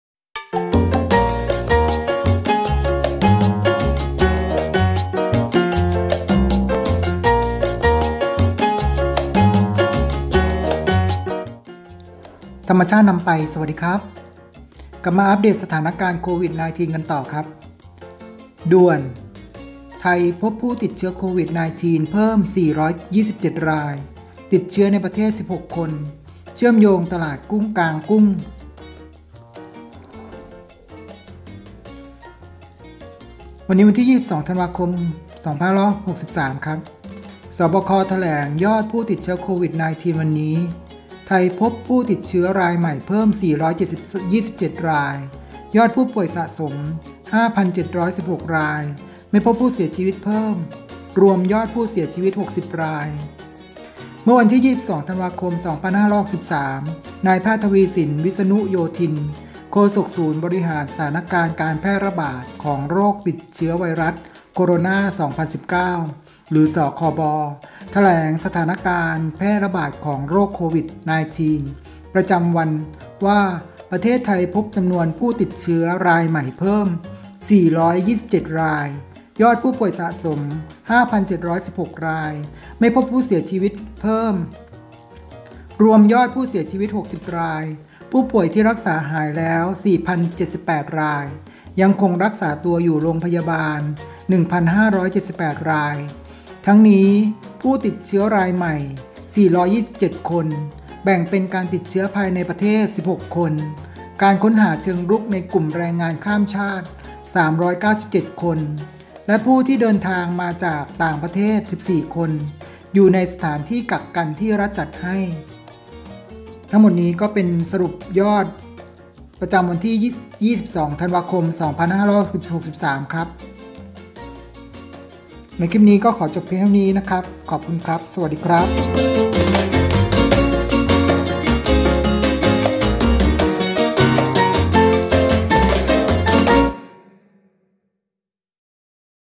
อัปเดทสถานการณ์ โควิด 19 22/12/63
ข่าวเด่น อัปเดทสถานการณ์ โควิด19 ยอดพุ่ง อัปเดทยอดผู้ติดเชื้อ โควิด 19 ประจำวันนี่ 22/12/56